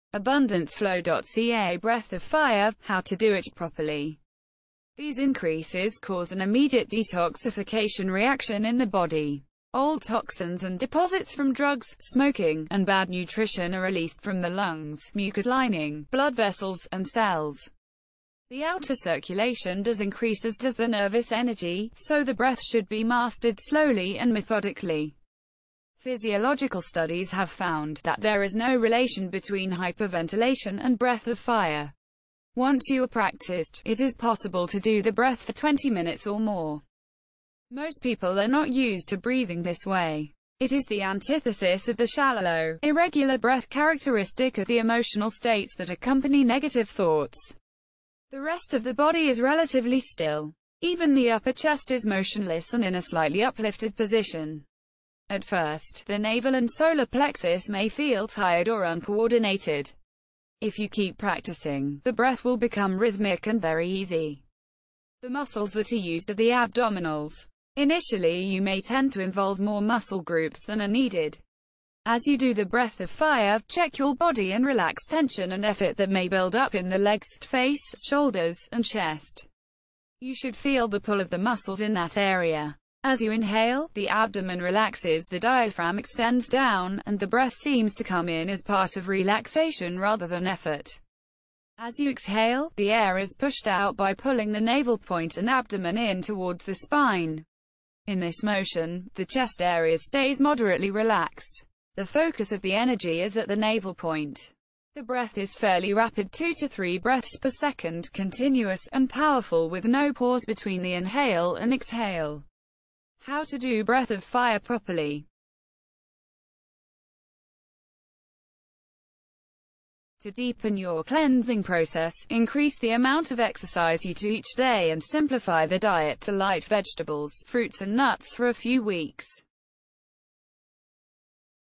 Kundalini Yoga: Breath Of Fire (part I) [Abundanceflow]
In this video Dhyan Navjot Kaur shares how to perform Kundalini Breath of Fire.
BEFORE you start practicing the exercises of this video, please follow this two VERY IMPORTANT steps:
1. Open your space with the Adi mantra. Please check the video on how to open here:
You will be receiving weekly useful Health, Yoga, Ayurveda Tips. Also, stories and invitations to different events geared to build community and connection.